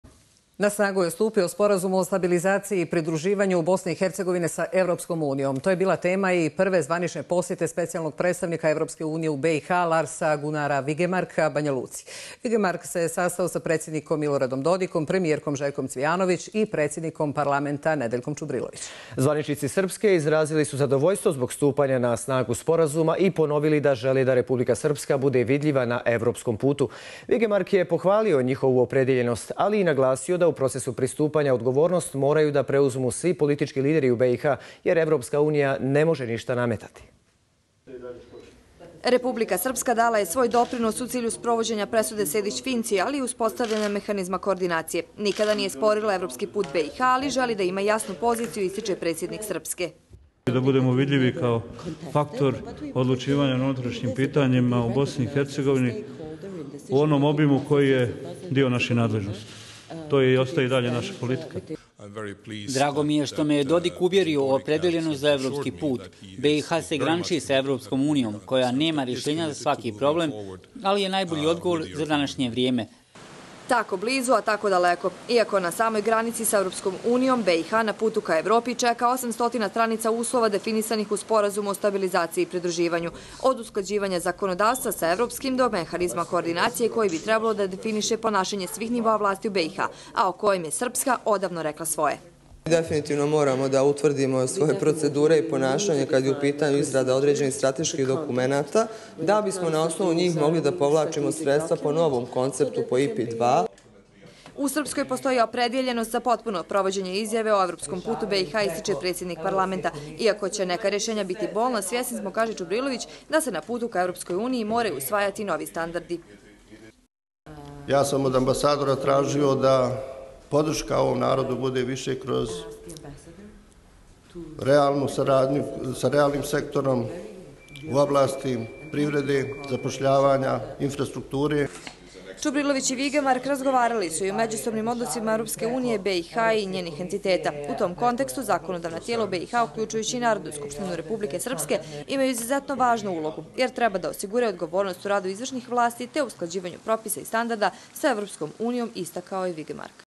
Vigemark sa zvaničnicima Srpske: Srpska želi biti vidljiva na evropskom putu
Predsjednik Republike Srpske Milorad Dodik izjavio je u Banjaluci da Republika Srpska u okviru svojih nadležnosti želi biti vidljiva na putu ka Evropskoj uniji.